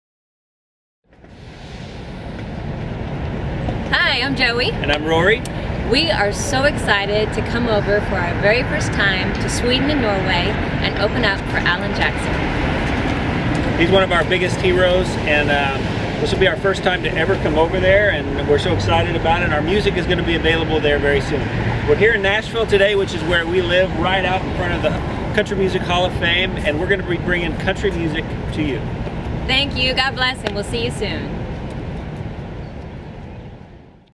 Joey+Rory's message to Norway and Sweden.
Joey+Rory will open up for Alan Jackson's concerts in Sweden and Norway
this summer 2011. It will be the greatest country happening in Scandinavia.
10 Aug  ESKILSTUNA, Vilsta - Sweden.  12 Aug  SKIEN, Stevneplassen and 13 Aug  STAVANGER - Forus Travbane in Norway.